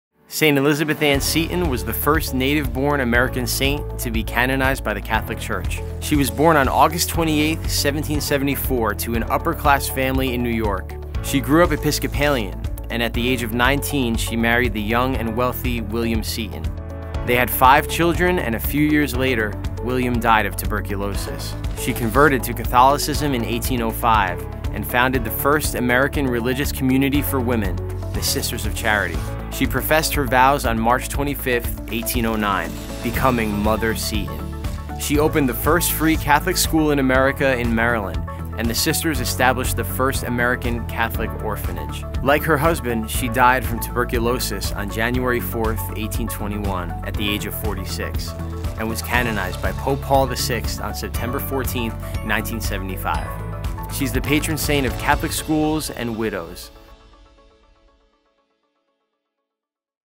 St. Elizabeth Ann Seton
In one minute learn about St. Elizabeth Ann Seton